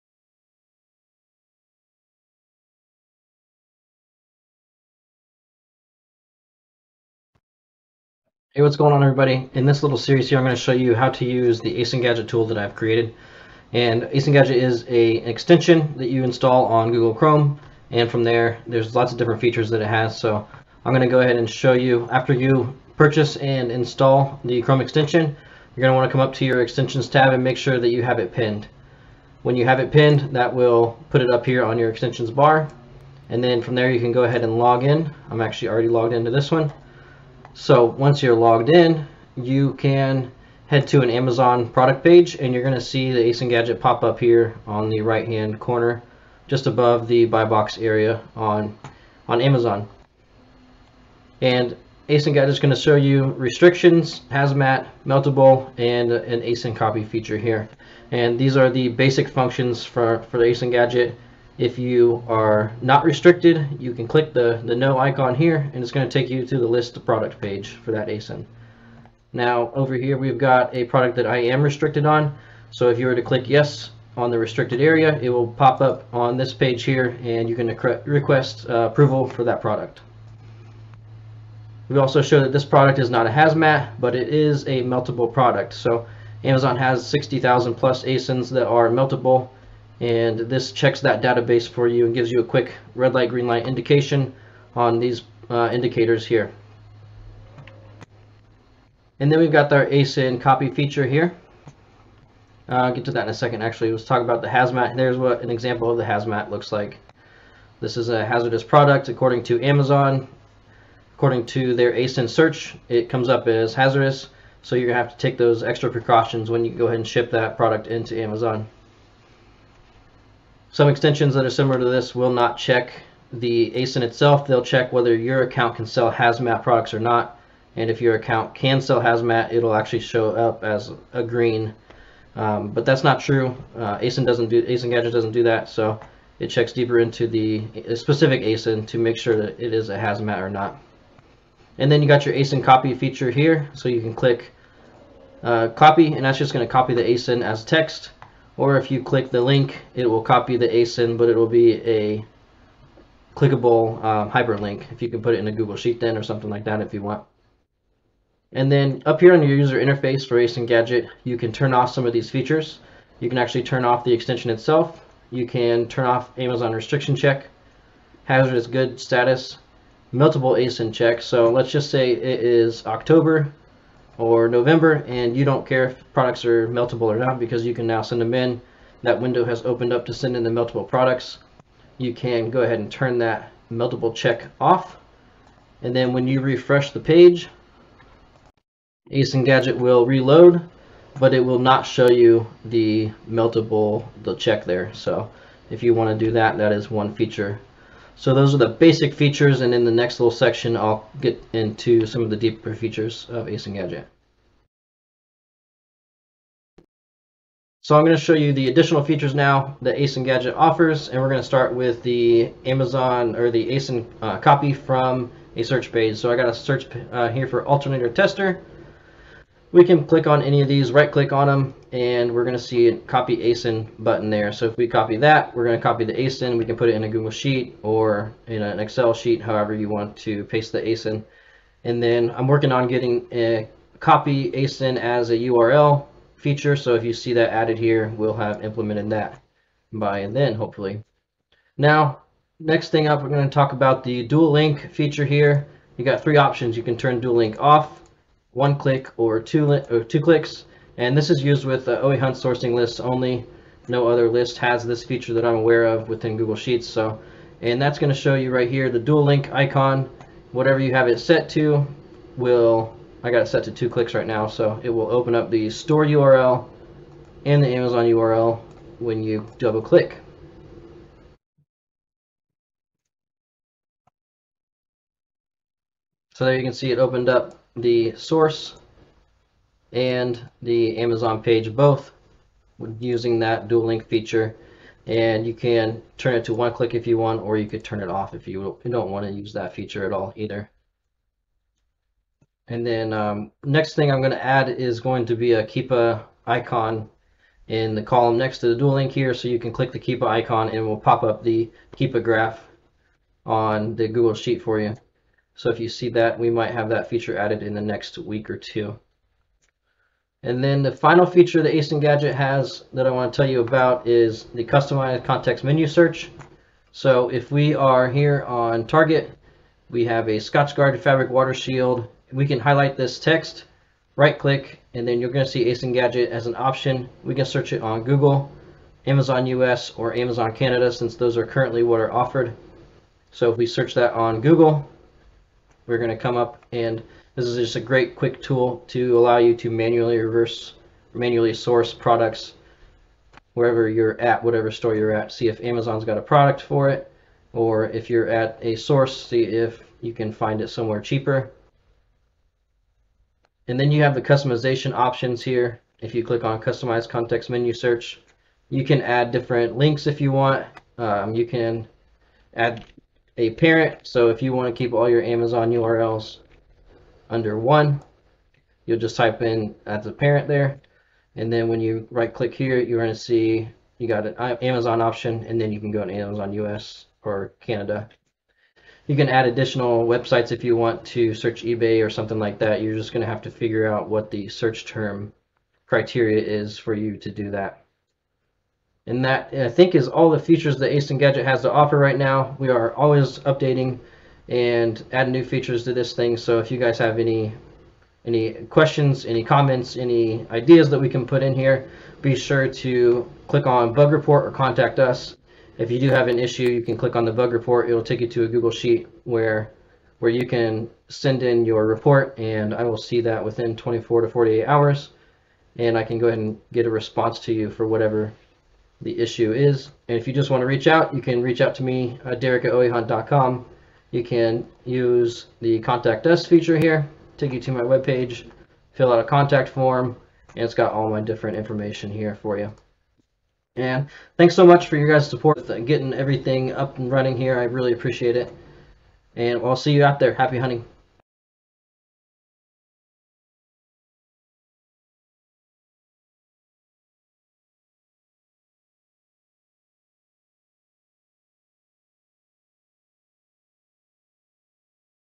ASIN Gadget Multi Tool Tutorial
In this video I'll cover all the features in my ASIN Gadget Google Chrome Extension multi-tool for Amazon Sellers!